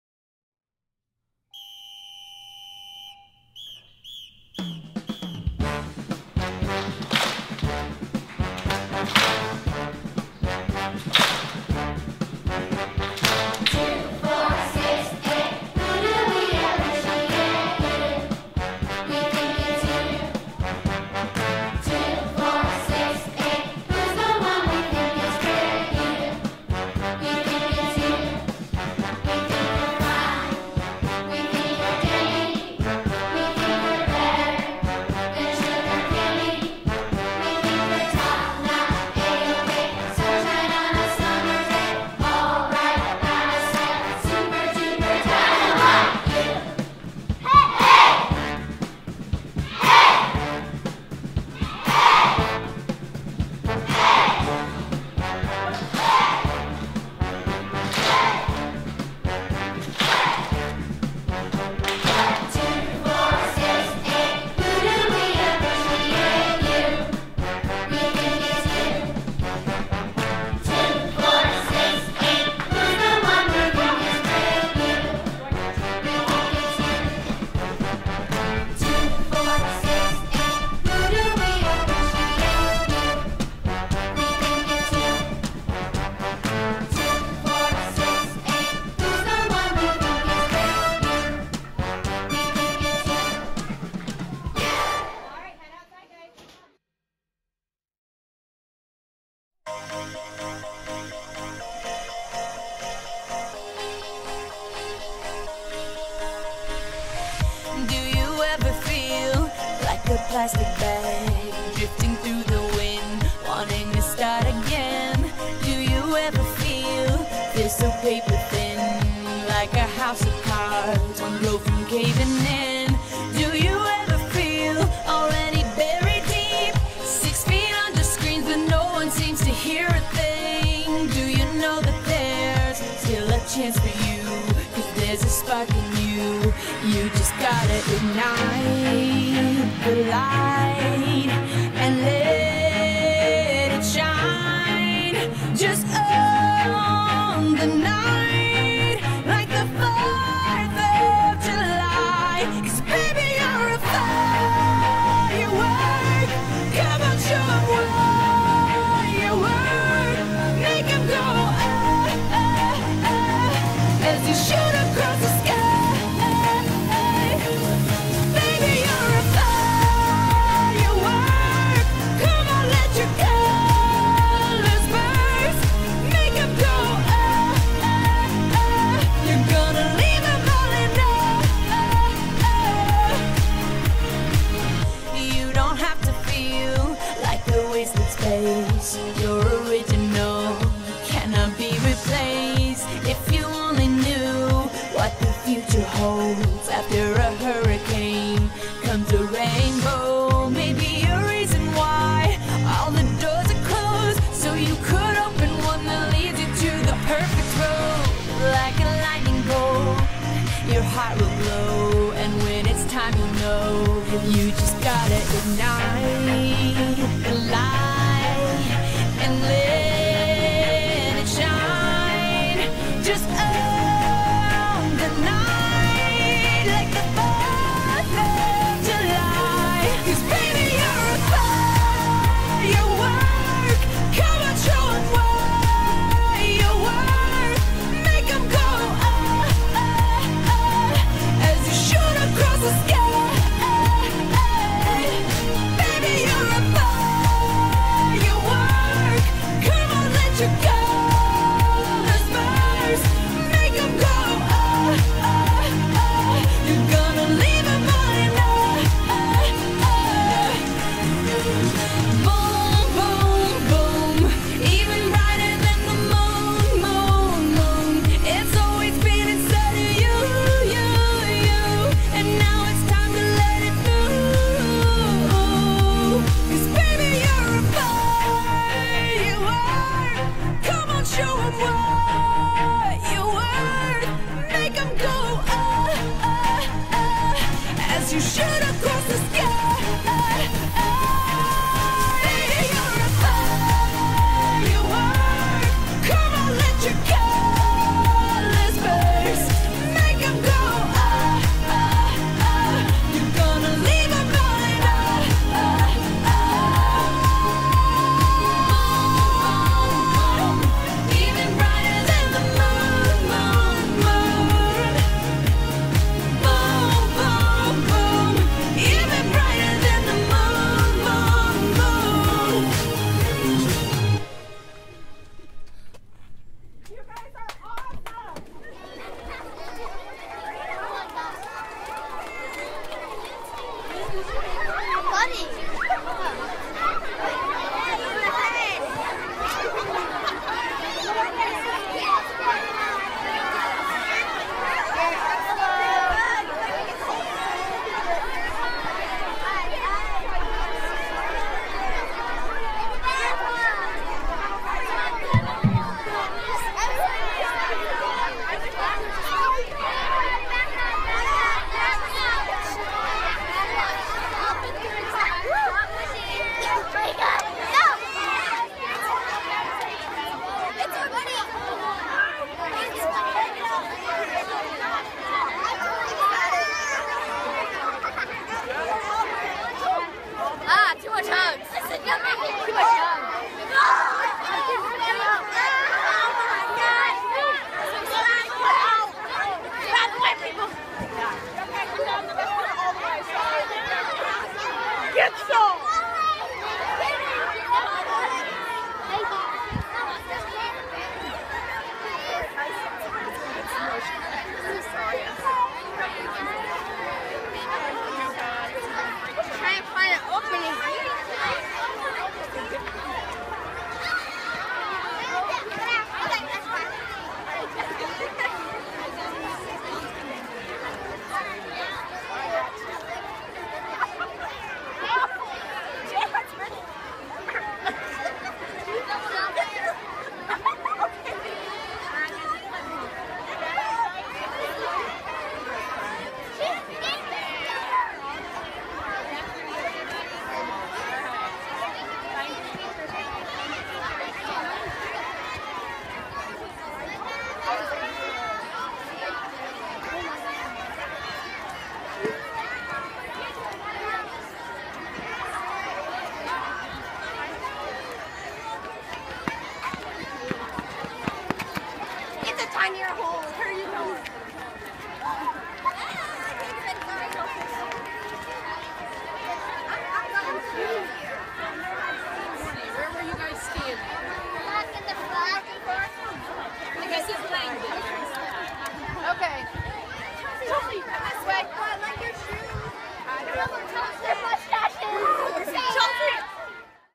Mrs. Crigler's Flashmob Farewell
John Weldon students surprised Mrs. Crigler with a flashmob to celebrate her retirement.